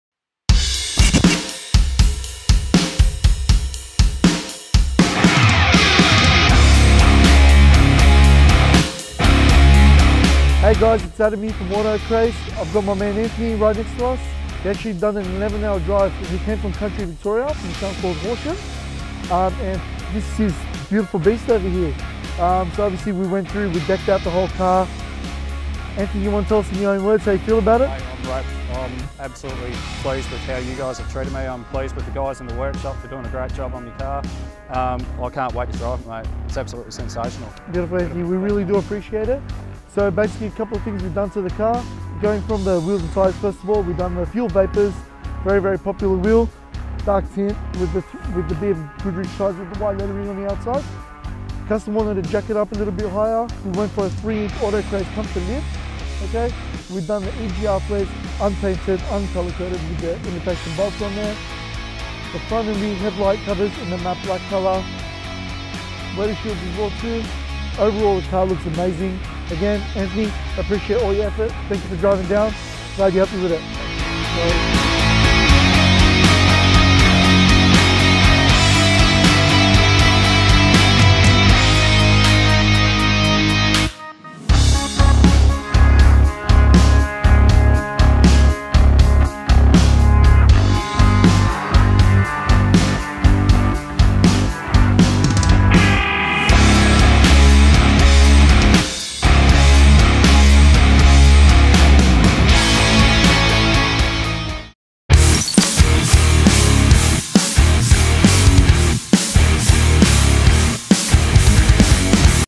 HORSHAM CRUSHER // Ford Ranger Wildtrak Upgrade Build - Australia 4WD Parts, Accessories & Wheels
HORSHAM CRUSHER // Ford Ranger by AutoCraze 2018

Straight from Horsham Victoria. Anthony spent 11 hours driving up to AutoCraze to get his Ranger decked out by the best.

Starting off his build, he went with a massive 3” AutoCraze lift kit. Rolling on a set of Fuel Vapor wheels wrapped in the famous BF Goodrich All-Terrain tyres.

Finishing off the build with a genuine OEM Ford nudge bar, weather shields, headlight and smoked led tail lights

We couldn’t be any happier with the result.

► Genuine Ford Nudge Bar
► Fuel Vapor Wheels
► BF Goodrich All-Terrain Tyres
► EGR Flares [Bolt-On Style]
► 3” AutoCraze Lift Kit
► Weather Shield
► Window Tint
► Head Light Trims
► Smoked LED Tail Lights

12 Months Interest Free Available